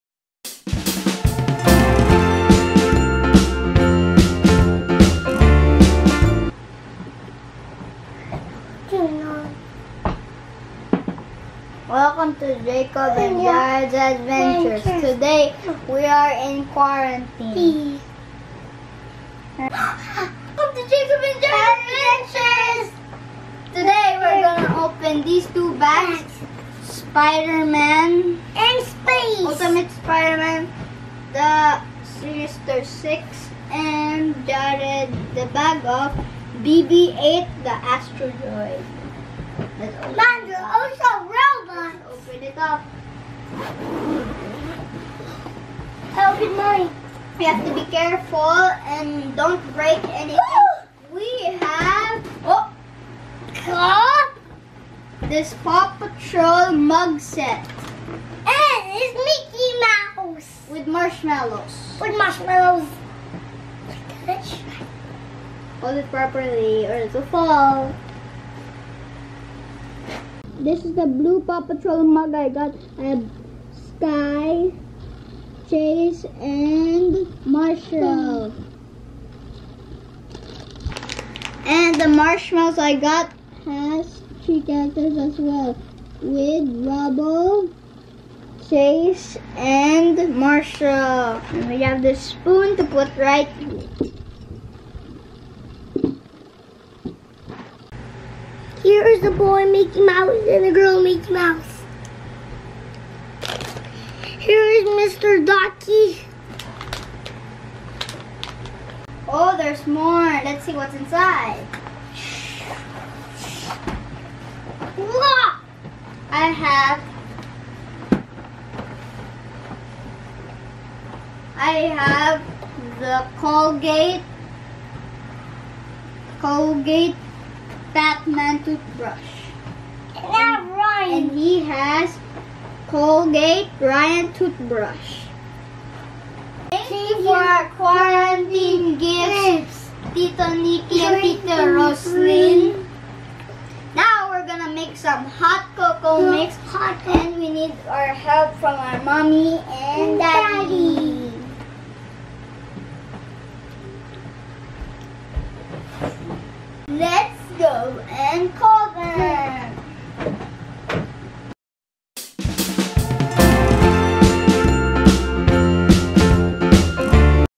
Receiving Gifts from our uncle and aunt
Jacob and Jared thanks their uncle and aunt for the surprise gifts. They also show what those gifts are.